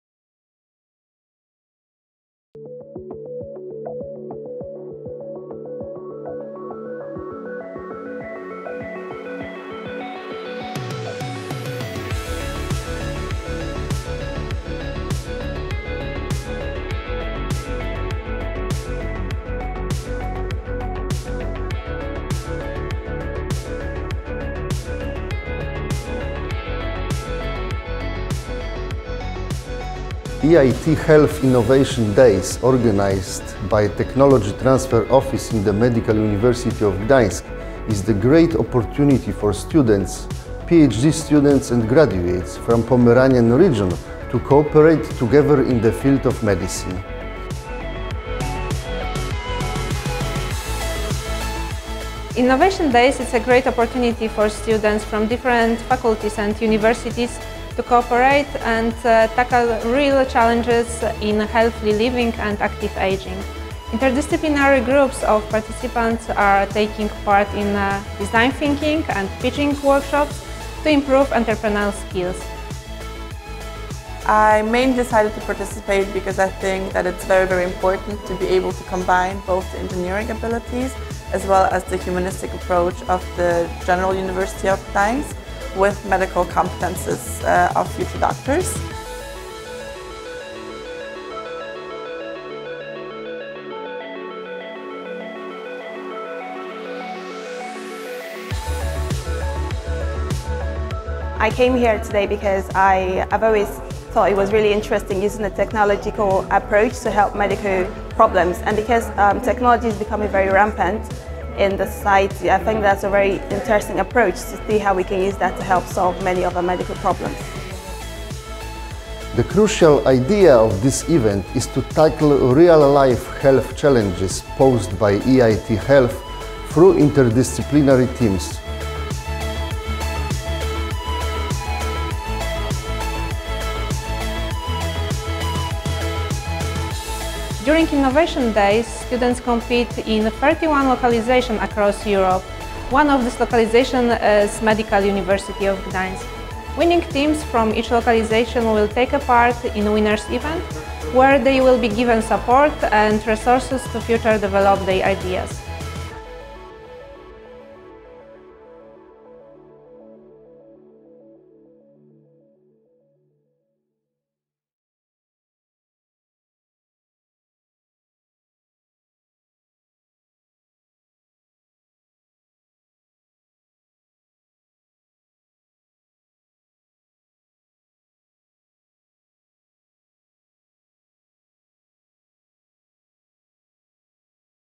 EIT Health Innovation Days 2019 at Medical University of Gdansk
28-29th of October 2019, Atheneum Gedanense Novum, Medical University of Gdansk.

The Innovation Days initiative involves both the academic environment of all Tri-city universities, including lecturers, students, graduates and PhD students, as well as the business ecosystem (start-ups, companies, entrepreneurship and social engineering trainers).

Film produced by Maj Film Produkcja Filmowa